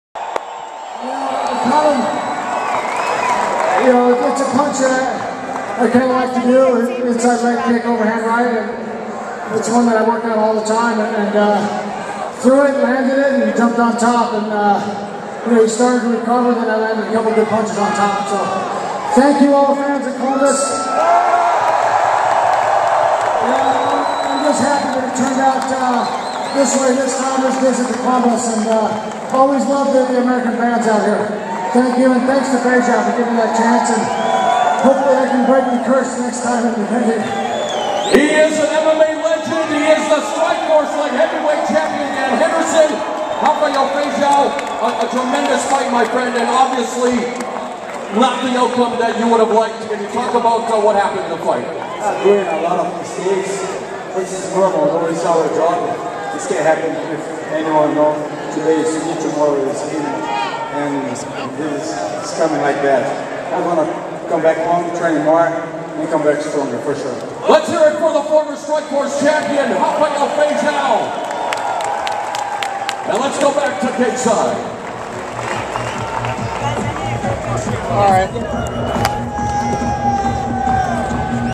Hendo Post-Fight in Strikeforce Cage
Check out this cageside look at Dan Henderson's post-fight interview and championship belt ceremony after winning the Strikeforce Light Heavyweight title on March 5, 2011. Taken by his cornerman following the 3rd round knockout of Rafael "Feijao" Cavalcante.